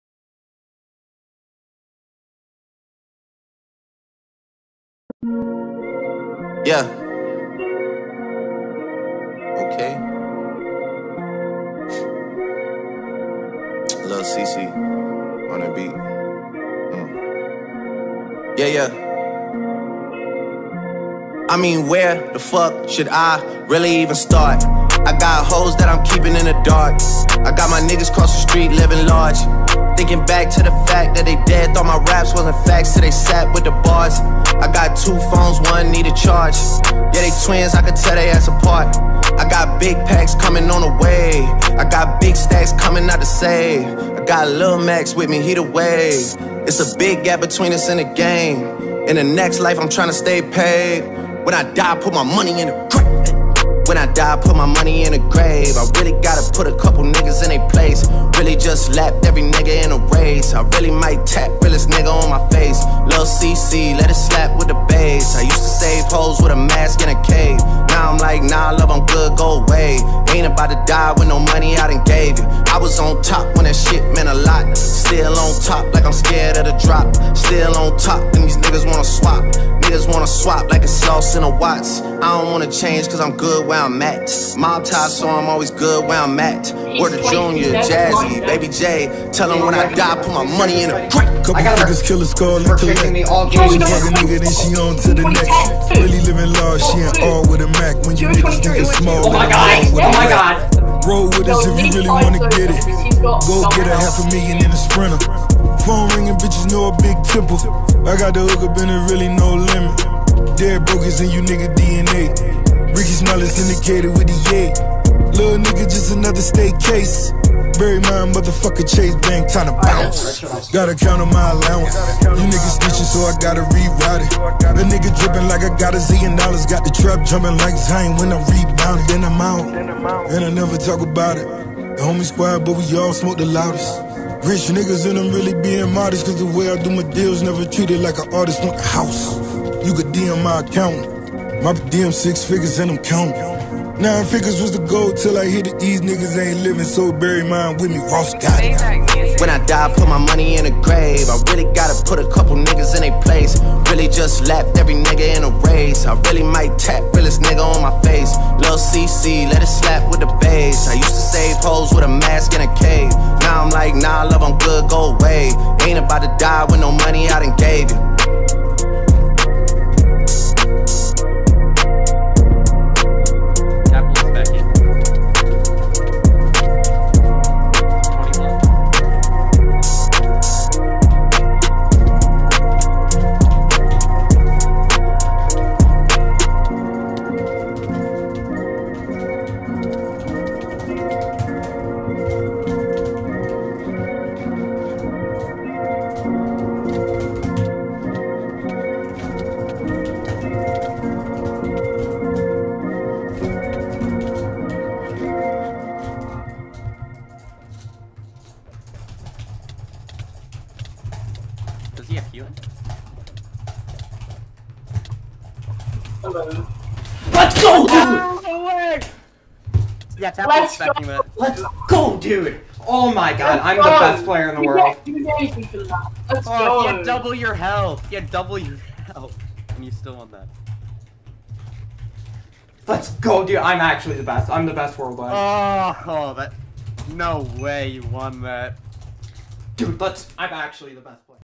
Tapple UHC #1: Best Clutch WW
for the last 20 minutes of the game I ran a track marathon around 0 0 evading every single player
GGs only